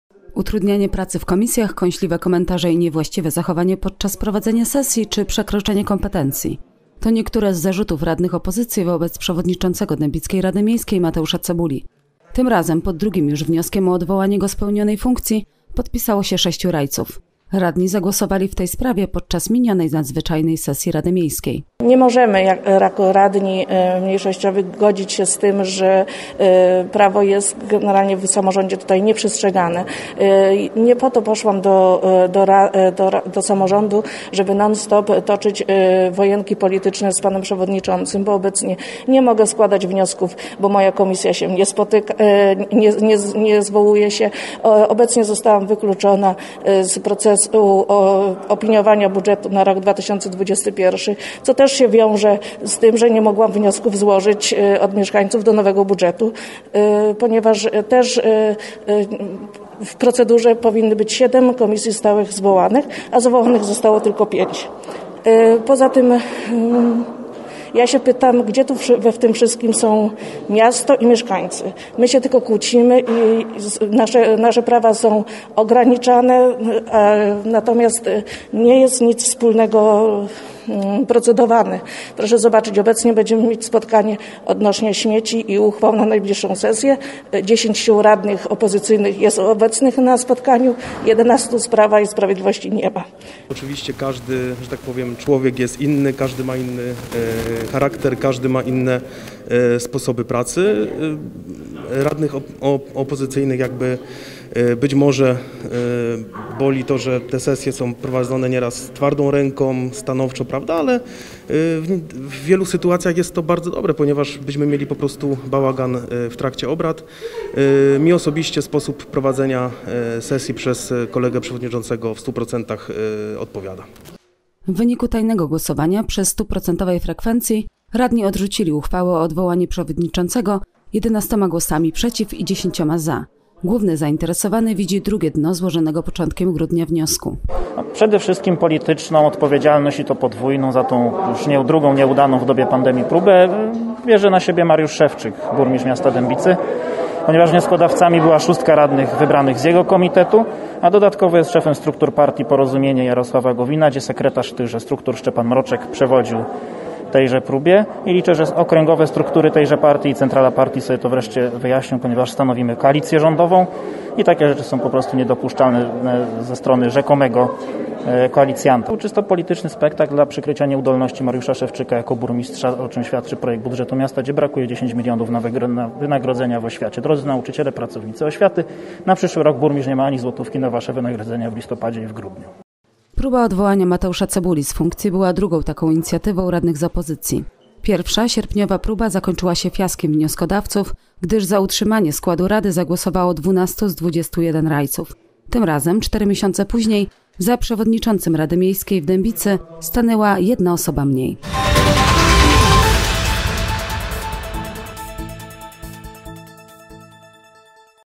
Radni z Pis obronili swojego przewodniczącego – Mateusz Cebula nadal na czele Rady Miejskiej w Dębic
Utrudnianie pracy w komisjach, kąśliwe komentarze i niewłaściwe zachowanie podczas prowadzenia sesji czy przekroczenie kompetencji. To niektóre z zarzutów radnych opozycji wobec przewodniczącego dębickiej Rady Miejskiej Mateusza Cebuli. Tym razem pod drugim już wnioskiem o odwołanie go z pełnionej funkcji podpisało się 6 rajców. Radni zagłosowali  w tej sprawie podczas minionej nadzwyczajnej sesji Rady Miejskiej.
W wyniku tajnego głosowania, przy 100% frekwencji, radni odrzucili uchwałę o odwołanie przewodniczącego 11 głosami przeciw i 10 za. Główny zainteresowany widzi drugie dno złożonego początkiem grudnia wniosku.
Próba odwołania Mateusza Cebuli z funkcji była drugą taką inicjatywą radnych z opozycji. Pierwsza, sierpniowa próba zakończyła się fiaskiem wnioskodawców, gdyż za utrzymanie składu rady zagłosowało 12 z 21 rajców. Tym razem, 4 miesiące później za Przewodniczącym Rady Miejskiej w Dębicy stanęła jedna osoba mniej.